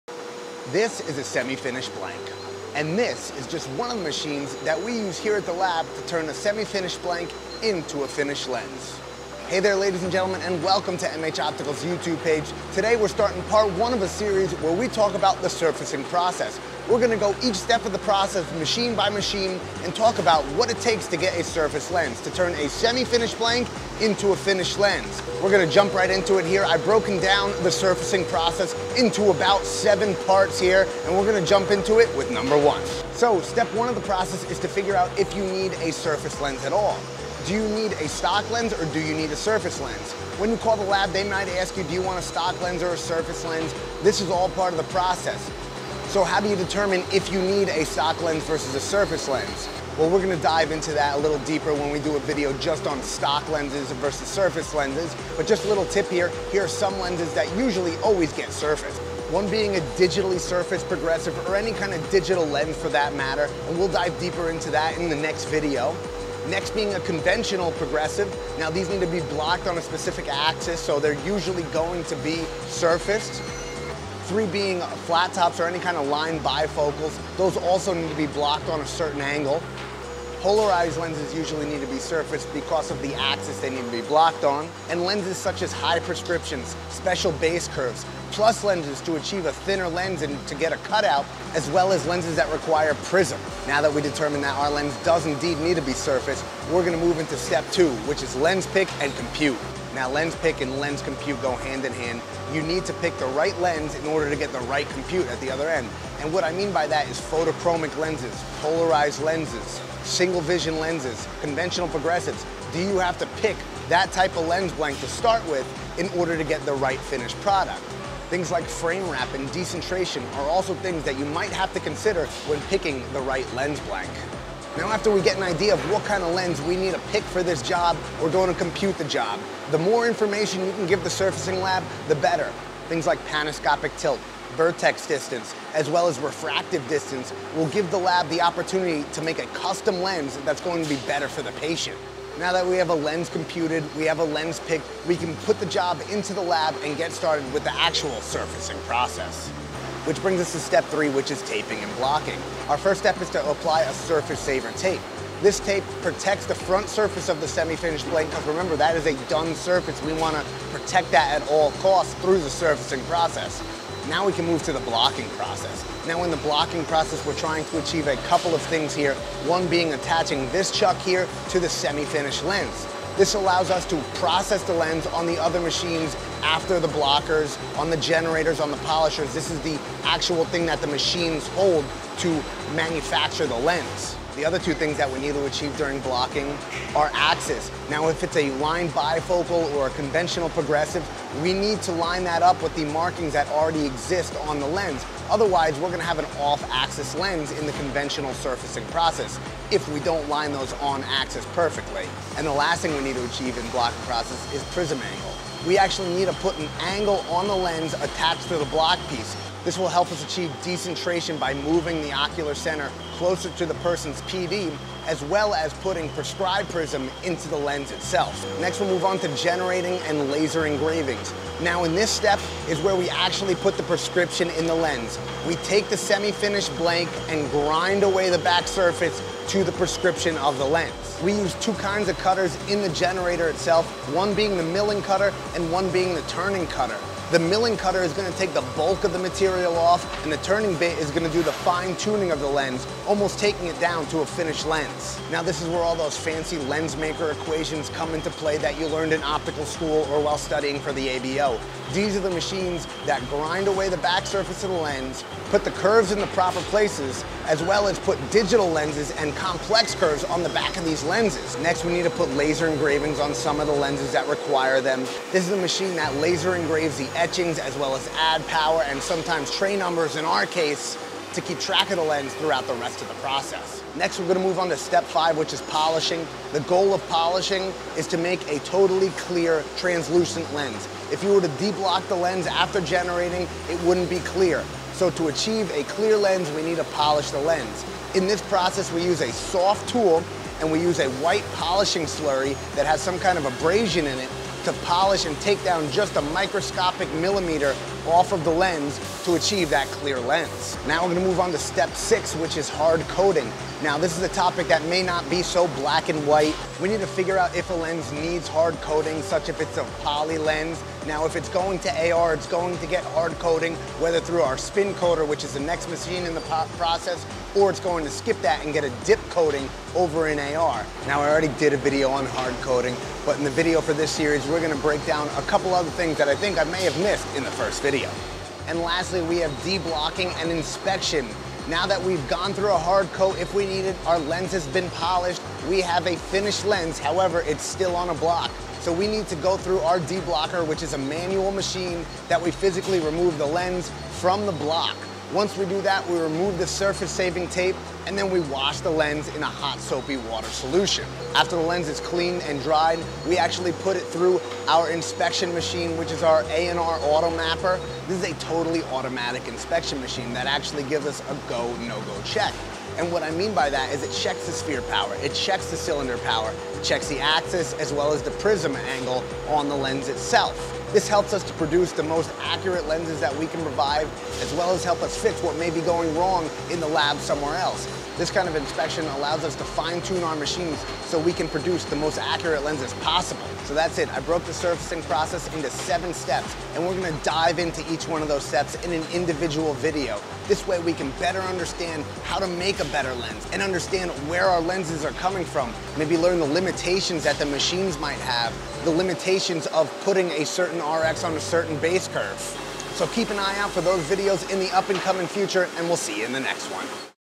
Lens Surfacing explained part 1!
Join us as we take a deeper dive into lens surfacing. This is part one of a series where we discuss all the steps in the surfacing process. We plan to highlight each part of the process and help to ensure the understanding of surfacing of a lens.
We have broken down the surfacing process into seven parts:

Part 1: Stock or surface?
Part 2: Lens pick/ compute
Part 3: Taping and blocking
Part 4: Generating and engraving
Part 5: Polishing
Part 6: Hard coating
Part 7: De-blocking/lens inspection